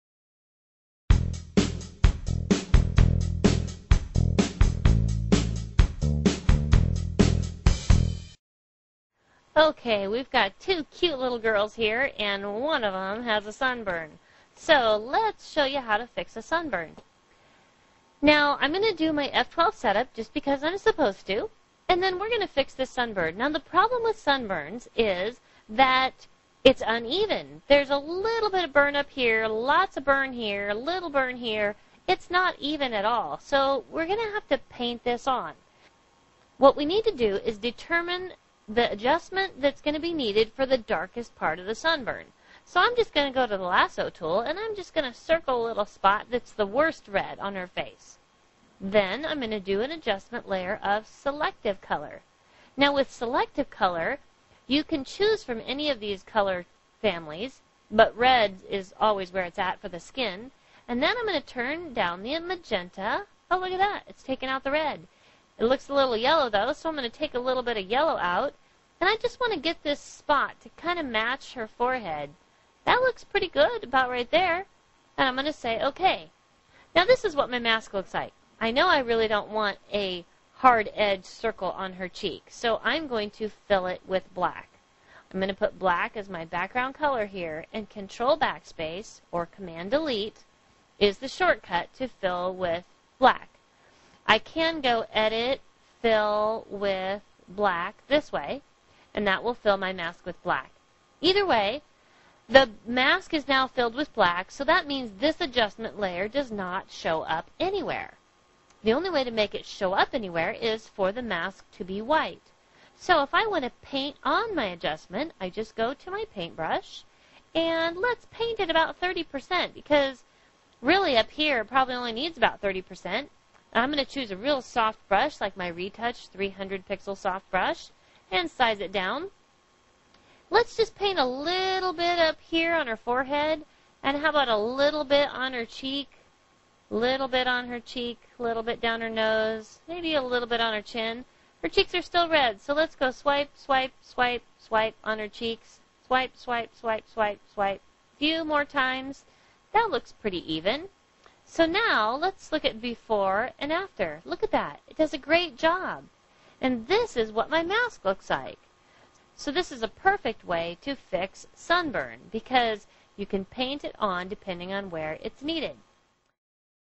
Photoshop sunburn Fix
Another clip from Suzette Allen's VSP video series #6. This instructional set of 12 VCD's is for the professional portrait and wedding photographer.